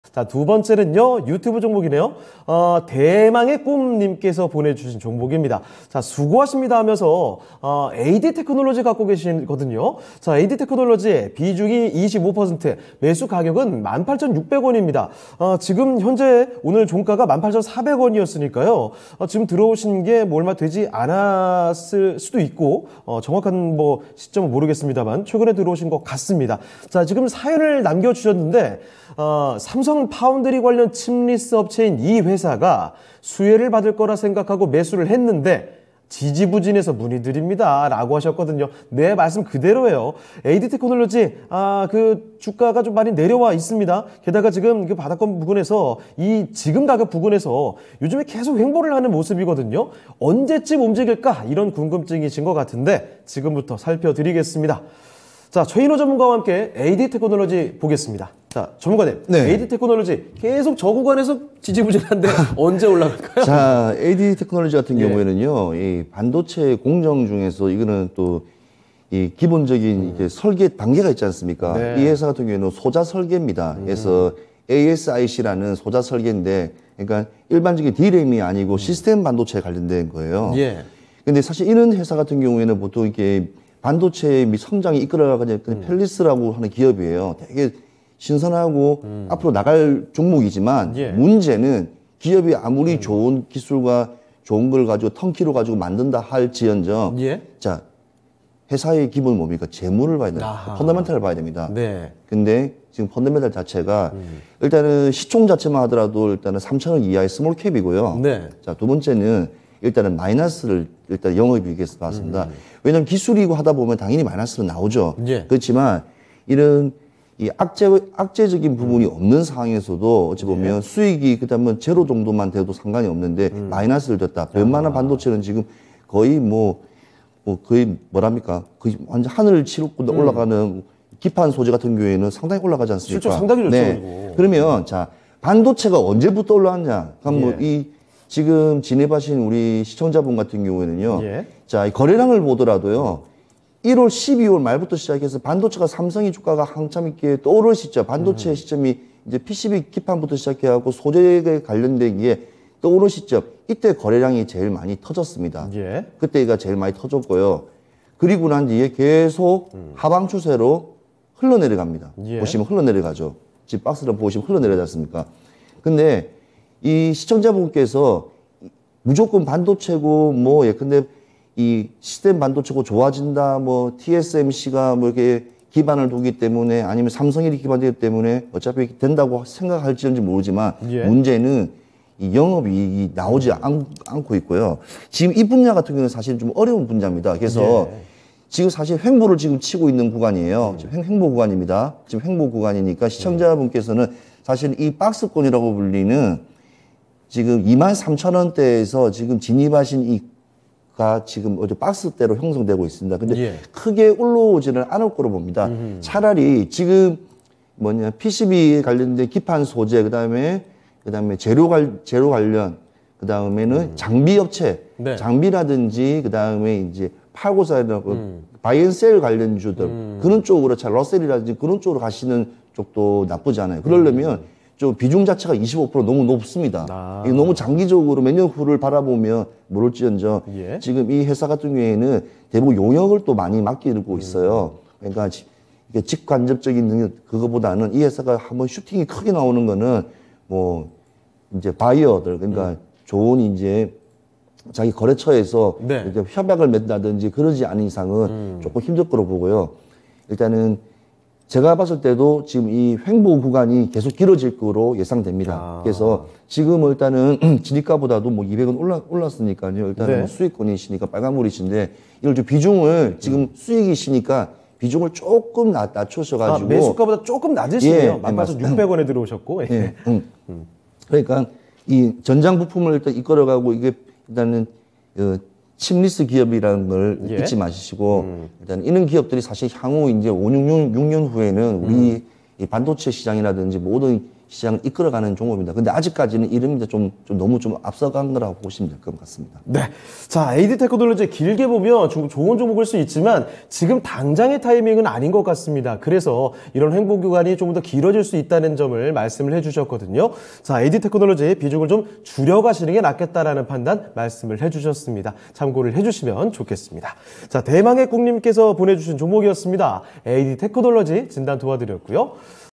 다른 반도체 잘가는데...홀로 마이너스 /에이디테크놀로지
에이디테크놀로지(200710) / 18,600원 / 25%

▶토마토TV_유튜브 채널: http: //
▶출연 : 최인오(토마토투자클럽)